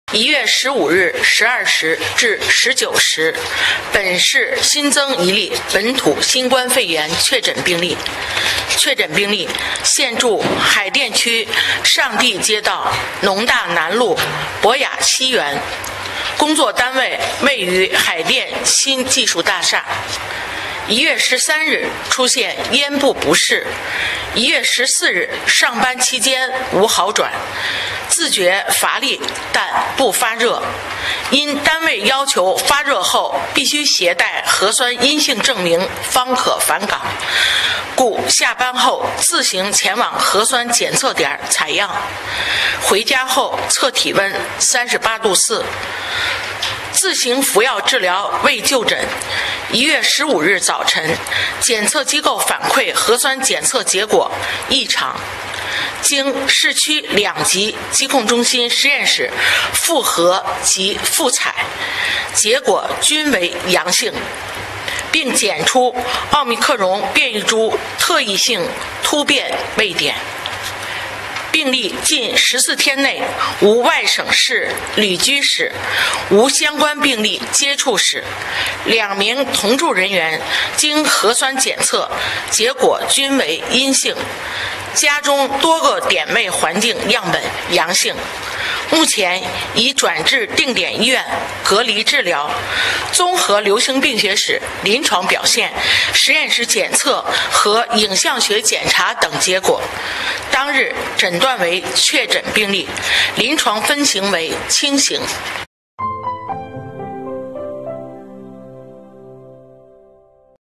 北京新增一例本土新冠肺炎确诊病例 为奥密克戎变异株阳性
1月15日，在北京市新型冠状病毒肺炎疫情防控工作第266场新闻发布会上，北京市疾病预防控制中心副主任、全国新型冠状病毒肺炎专家组成员庞星火介绍，1月15日19时29分，北京市确诊新增1例本土新冠肺炎病例。
        确诊病例现住海淀区上地街道农大南路博雅西园，工作单位位于海淀新技术大厦。1月13日出现咽部不适，1月14日上班期间无好转，自觉乏力但不发热，因单位要求发热后必须携带核酸阴性证明方可返岗，故下班后自行前往核酸检测点采样，回家后测体温38.4℃，自行服药治疗，未就诊，1月15日早晨检测机构反馈核酸检测结果异常，经市区两级疾控中心实验室复核及复采，结果均为阳性，并检出奥密克戎变异株特异性突变位点。病例近14天内无外省市旅居史，无相关病例接触史，2名同住人员经核酸检测，结果均为阴性。家中多个点位环境样本阳性。目前已转至定点医院隔离治疗，综合流行病史、临床表现、实验室检测和影像学检查等结果，当日诊断为确诊病例，临床分型为轻型。